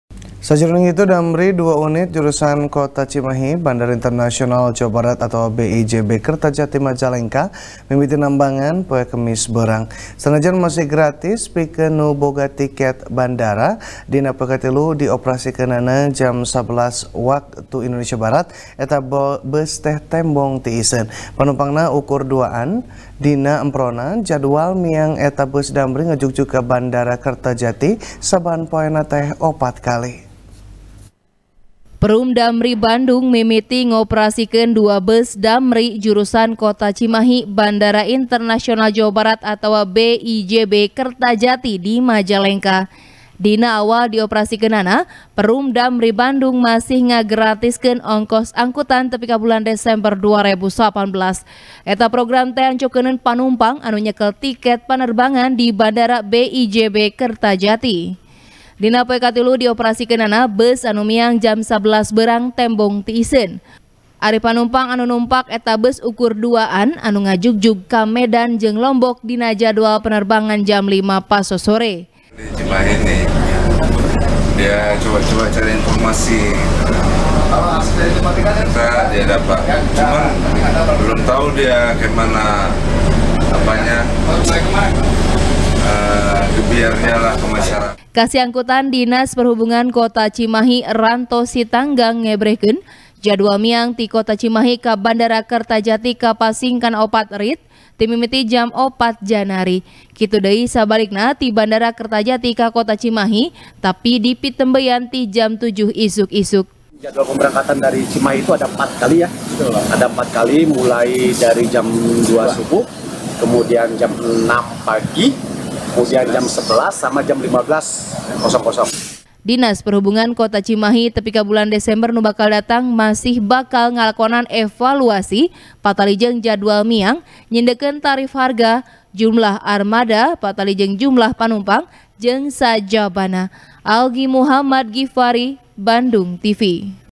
Cimahi Nataharkeun Damri Jurusan Cimahi-Bandara Kertajati | TP | BANDUNGTV
TP | BANDUNGTV | DERI FIRMAN | Damri Dua Unit Jurusan Kota Cimahi – Bandara International Jawa Barat Atawa B-I-J-B Kertajati Majalengka, Mimiti Nambangan, Poe Kemis Beurang,, Sanajan Masih Gratis Pikeun Nu Boga Tiket Bandara, Dina Poe Katilu Dioperasikeunana Jam Sabelas Waktu Indonesia Barat, Eta Beus Tembong Tiiseun,, Panumpangna Ukur Duaan,, Dina Emprona, Jadwal Miang Eta Beus Damri Ngajugjug Ka Bandara Kertajati Saban Poena Teh Opat Kali,,